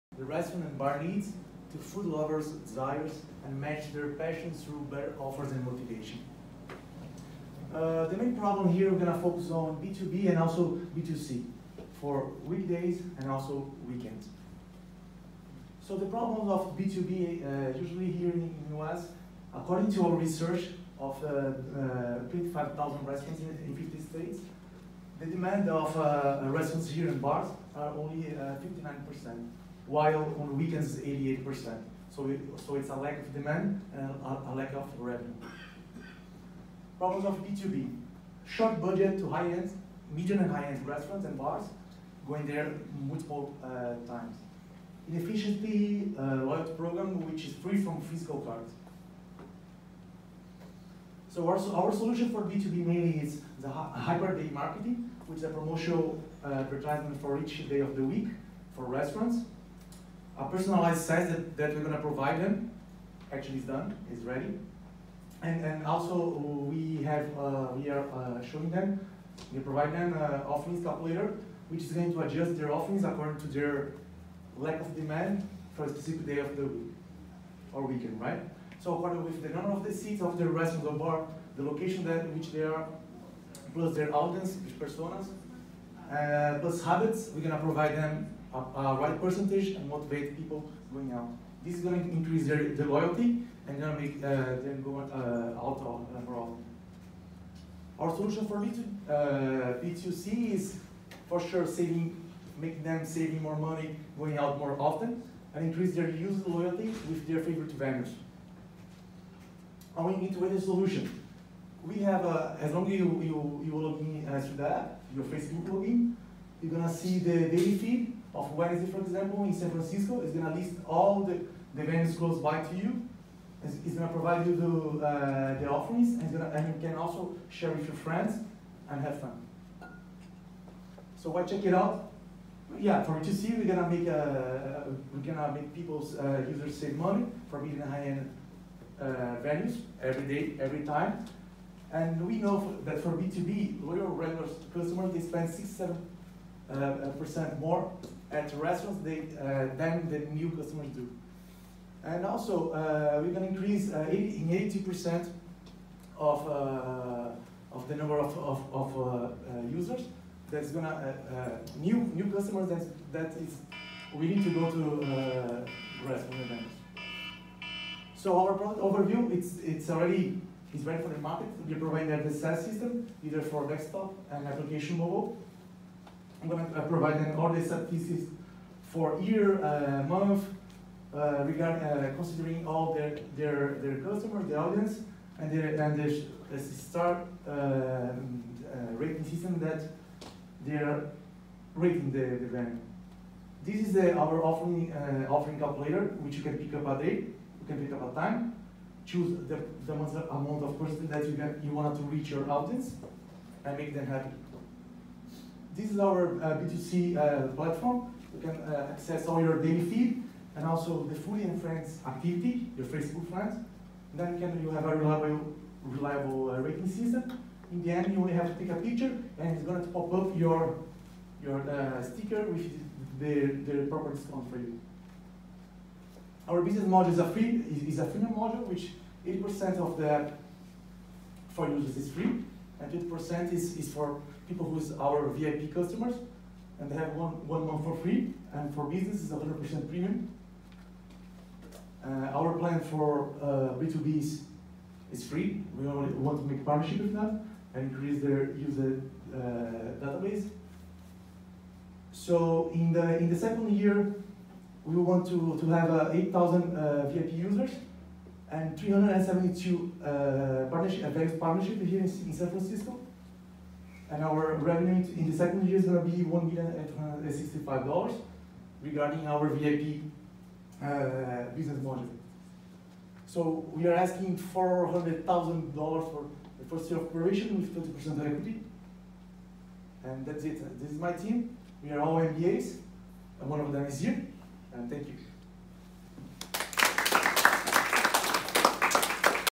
Foreign Startups Pitch: CHECKITOUT! by Felipe Souza of BRAZIL
Foreign Startups Mixer n Pitch event at RocketSpace in San Francisco Oct 24, 2018. Startup Founder Pitch of CheckItOut! by Felipe Souza of Brazil